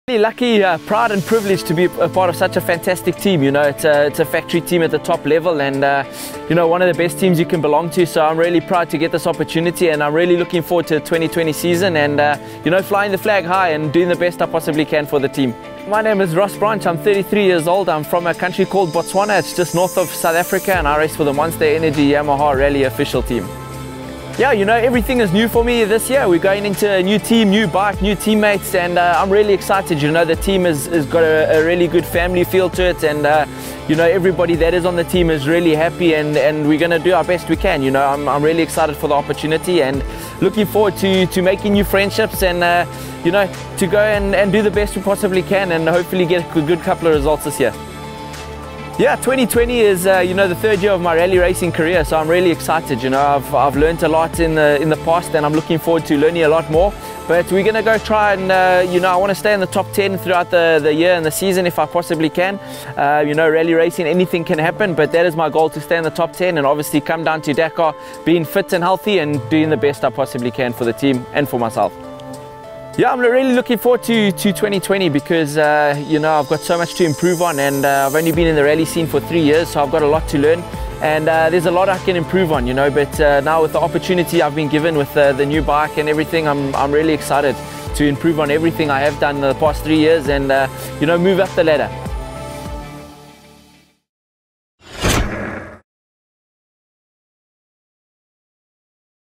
Ross Branch Joins Monster Energy Yamaha Rally Official Team
Ross Branch will line-up in the Monster Energy Yamaha Rally Official Team this year, arriving as Botswana’s first stage winner at the 2020 Dakar Rally and top rookie in the 2019 edition of the event. The 33-year-old has also shown great speed during his FIM Cross-Country Rallies World Championship outings and is aiming for further improvement in 2020.